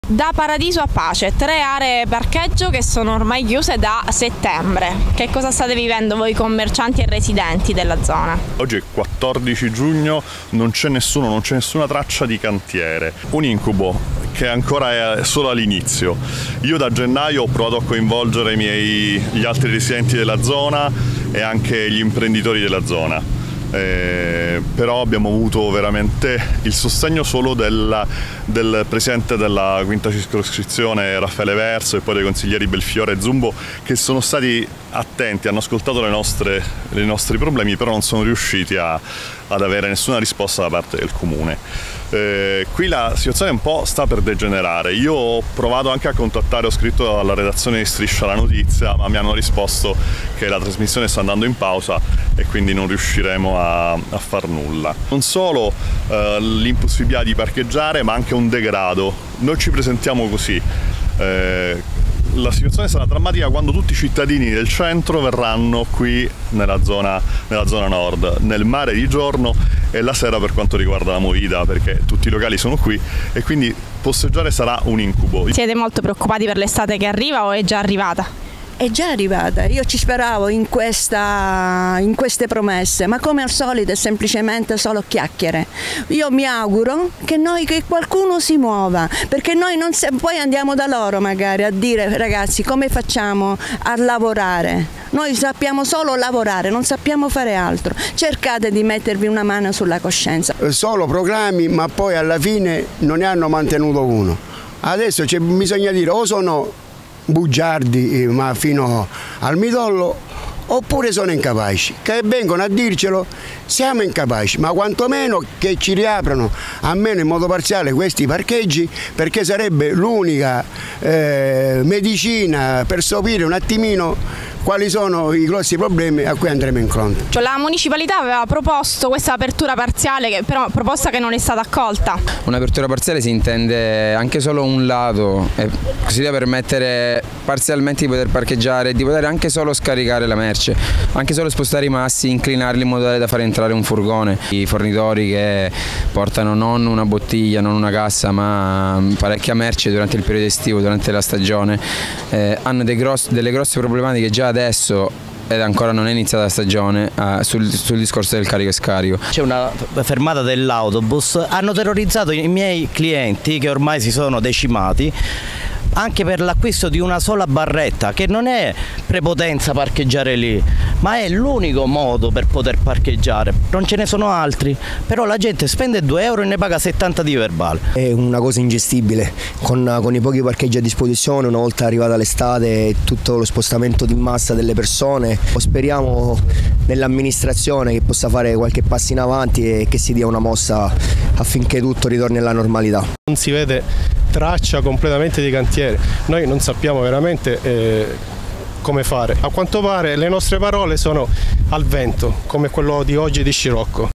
Parcheggi chiusi da Paradiso a Pace. La protesta dei commercianti: "L'estate sarà invivibile"
Il comitato di residenti e imprenditori della zona chiede almeno un'apertura parziale in attesa dell'inizio dei lavori